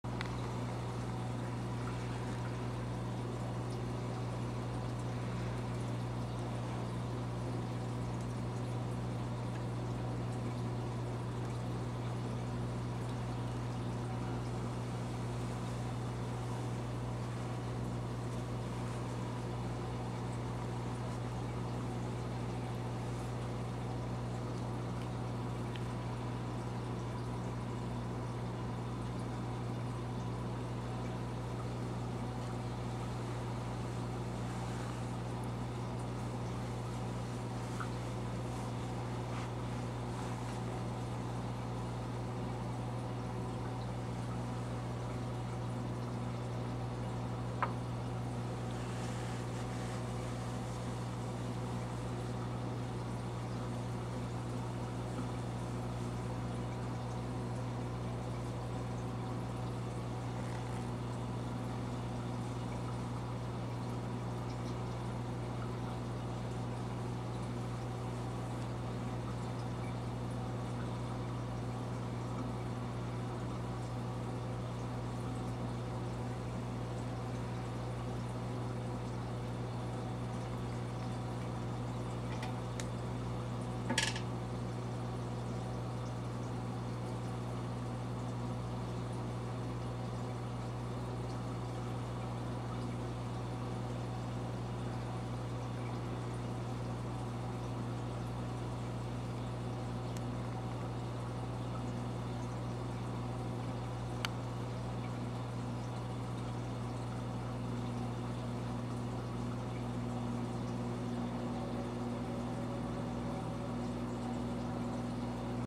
RBTA feed 102614
Feed the RBTA a half of a silver-side.